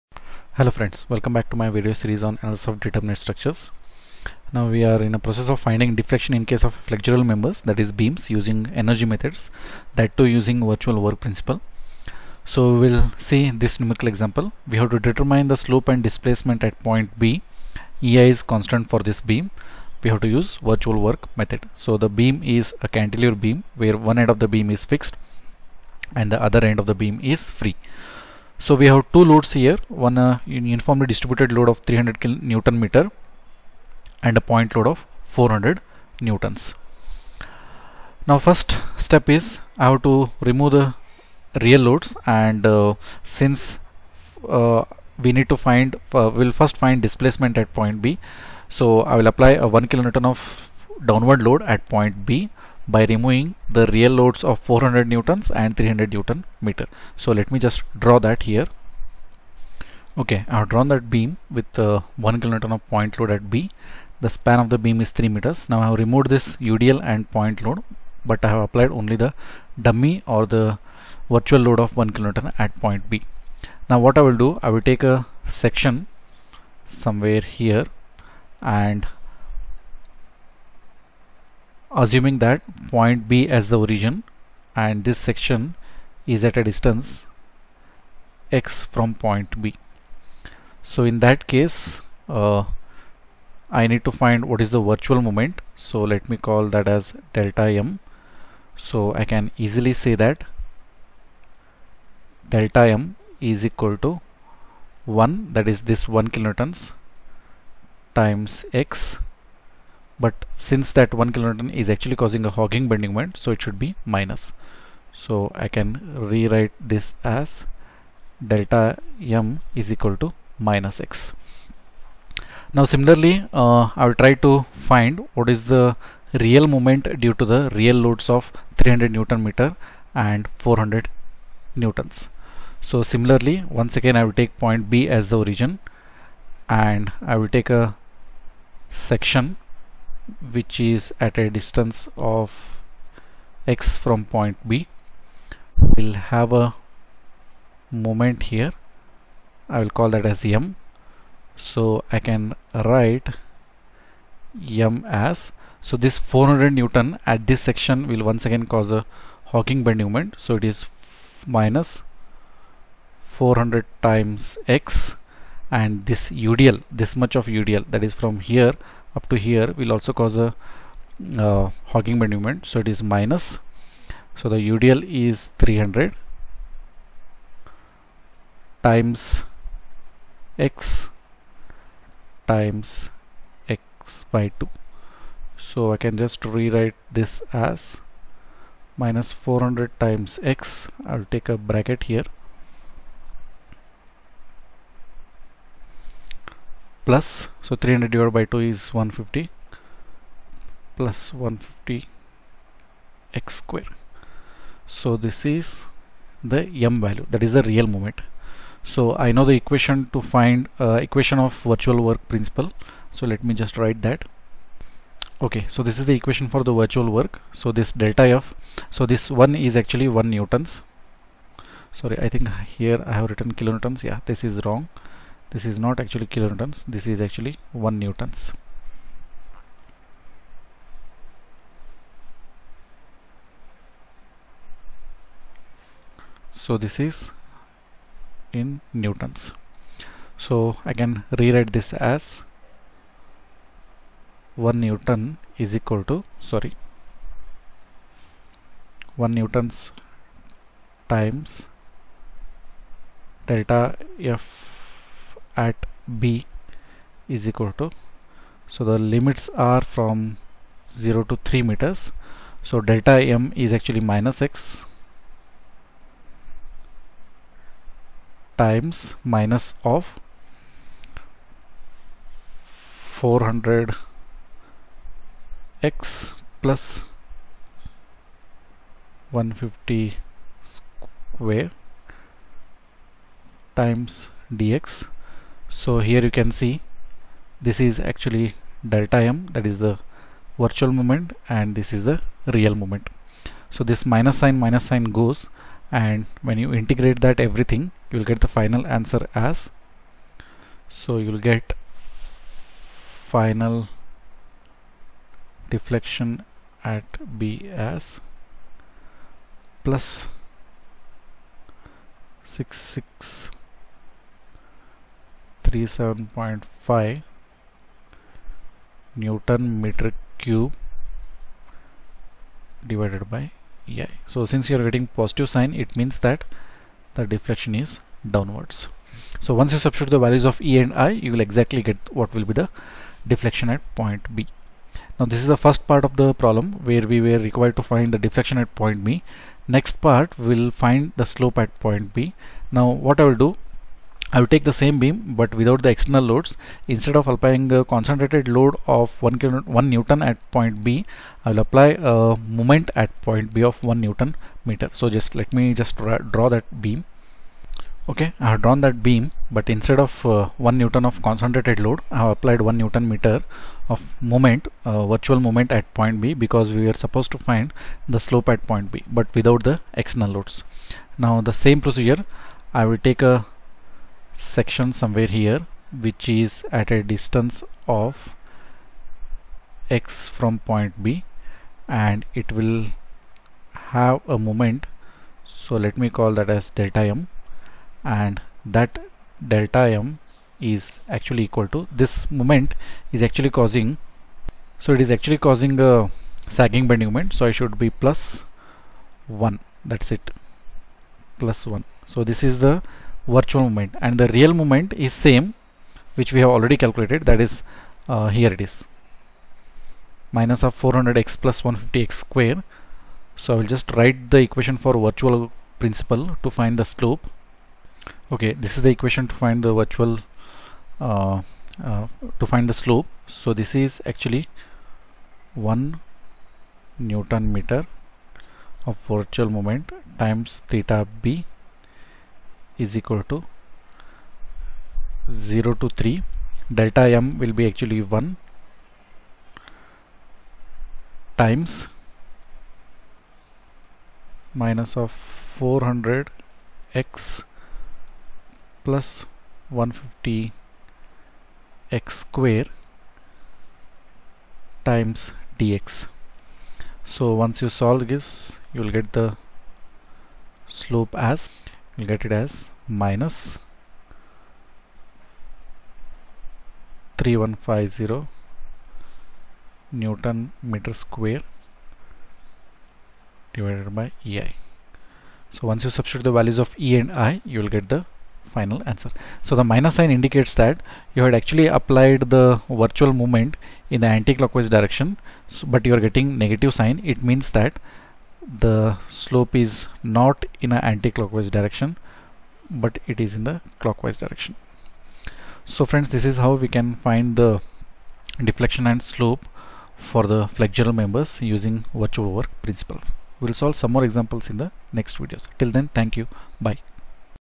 Virtual Work Principle-Deflection in Determinate Beams-Example 3
In this video, I have explained how to find the deflection of determinate beams using the Virtual work Principle.